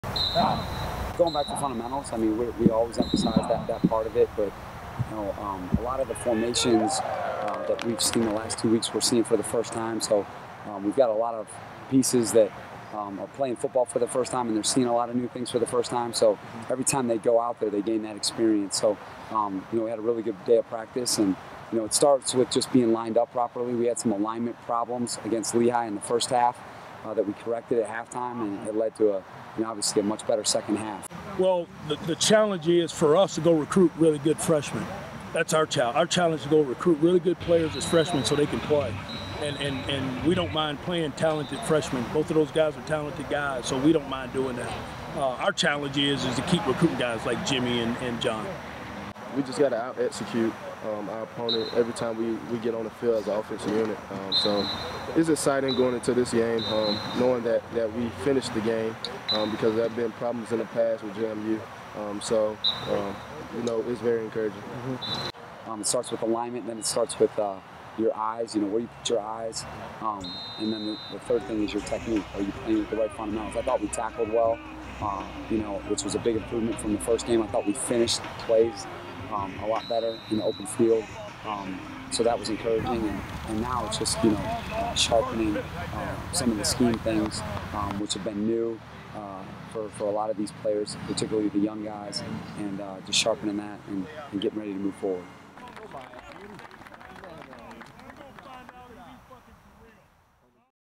Football Update: JMU vs. St. Francis
We head over to practice to hear what Head Coach Withers and Defensive Coordinator Brandon Staley have to say about JMU's first home game this saturday against St. Francis